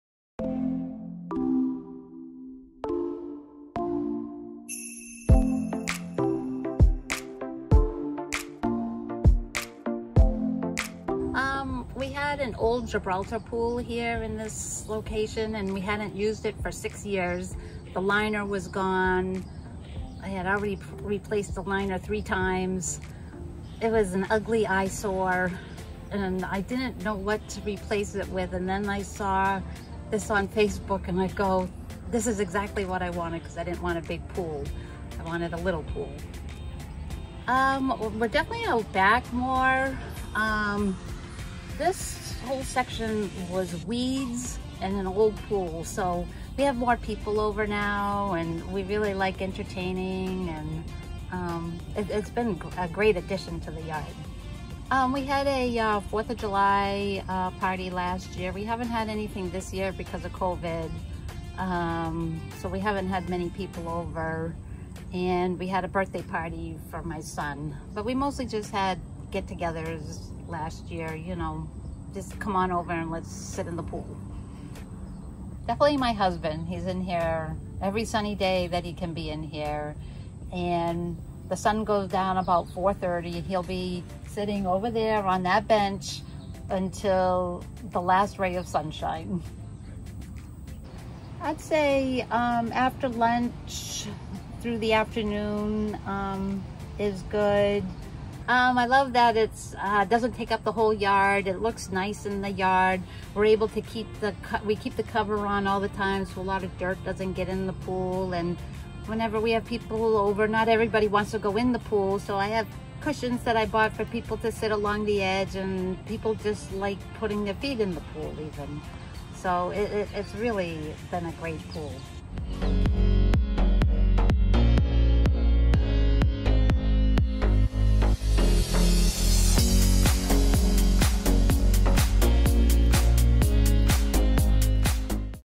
Stasia Dion: From Above Ground to Soake Pool
Stasia Dion discusses the major changes that happened in her yard and how it has improved it tremendously due to the addition of a Soake Pool.